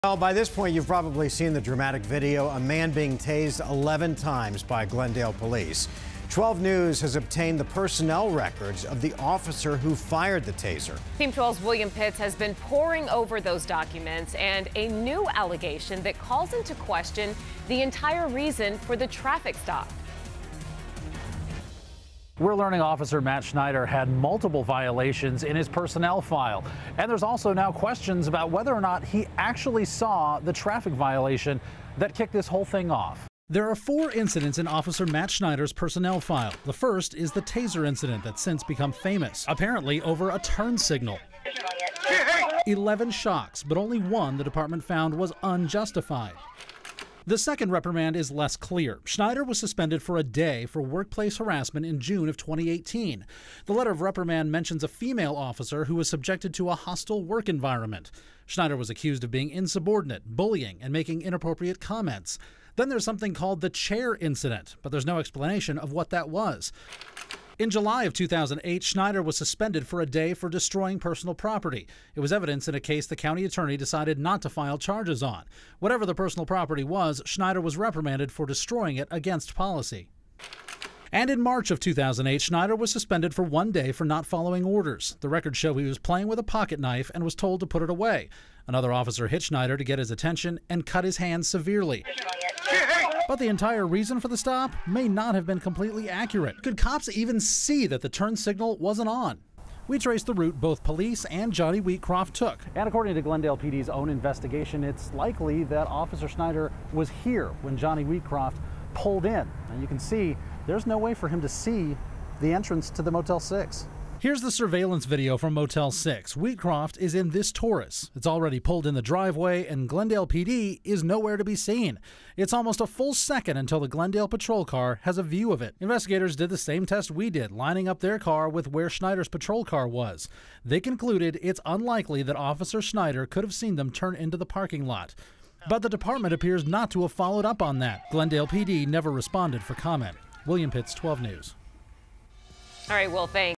Traffic stop called into question in Glendale PD tasing case
12 News has obtained the personnel file for Officer Schnieder, the Glendale police officer who tased a man 11 times during a traffic stop. New allegations call into question the entire reason for the traffic stop.